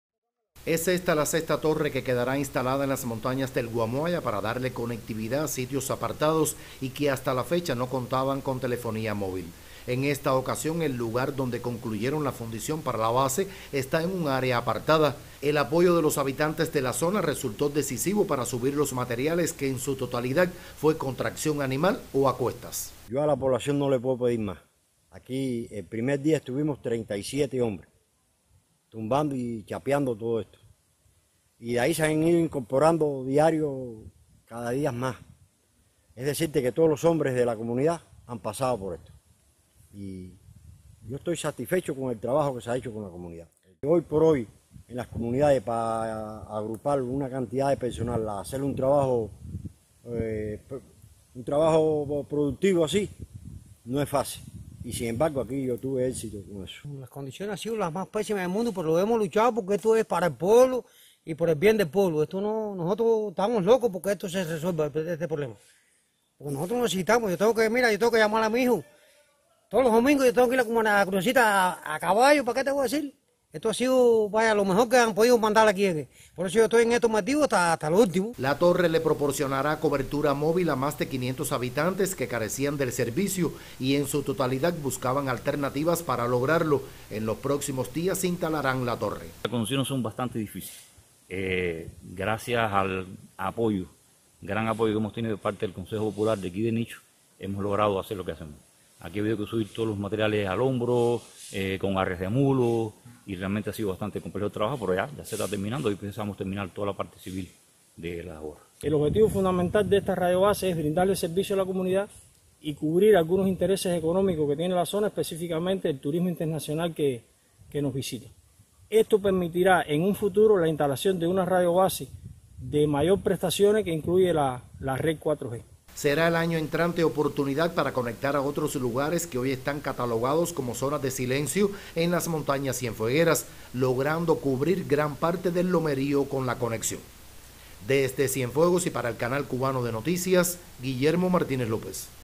Instalan nueva torre para telefonía celular en montañas de Cienfuegos
Integrantes de la brigada de montaje de ETECSA en Cienfuegos, concluyeron el proceso de fundición para instalar una torre que conecta la telefonía celular a la comunidad montañosa de El Nicho. Por la difícil ubicación geográfica de ese poblado, resultó decisiva la colaboración de sus habitantes en el traslado de los materiales hacia el lugar.